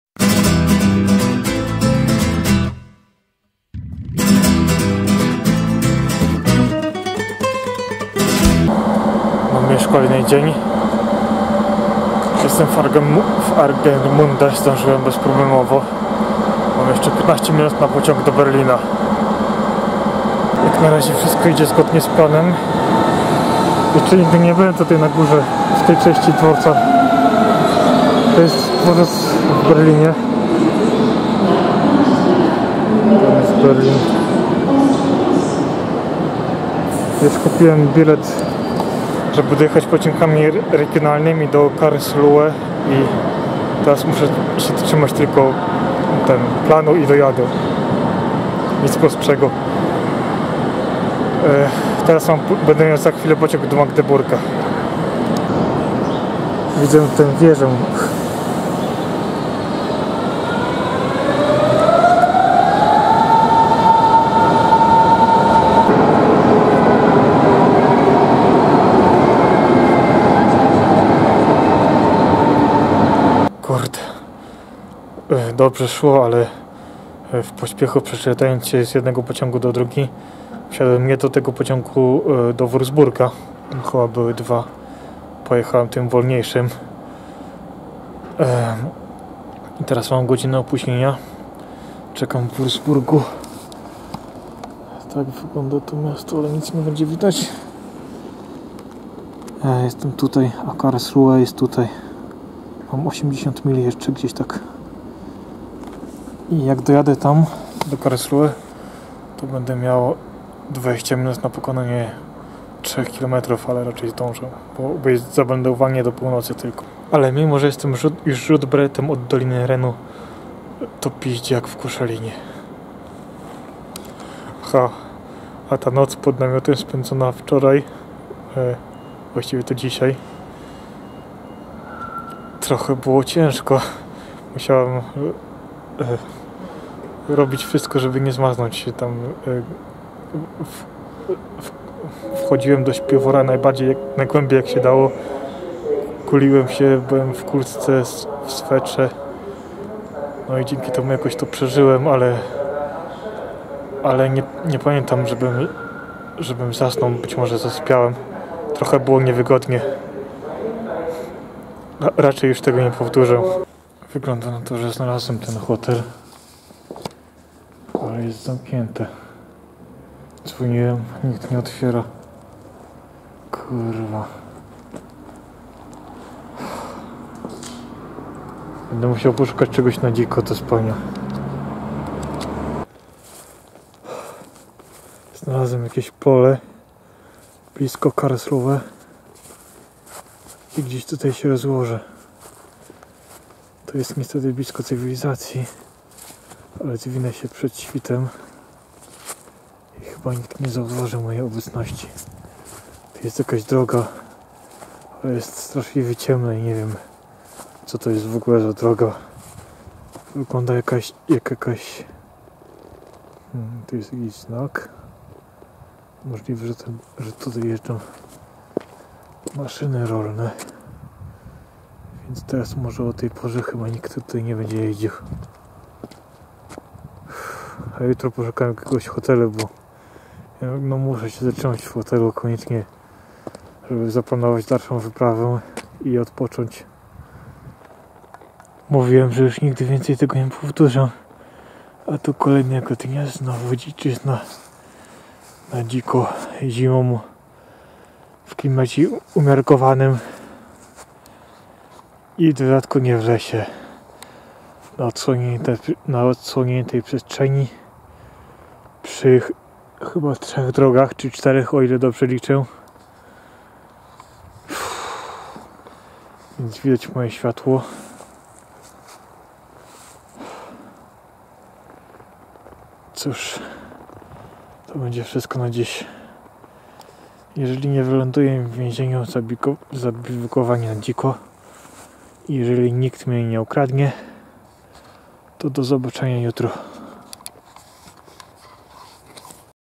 Wyprawa zimowa, odcinek 3
Pod koniec grudnia dotarłem do Karlsruhe, a tam...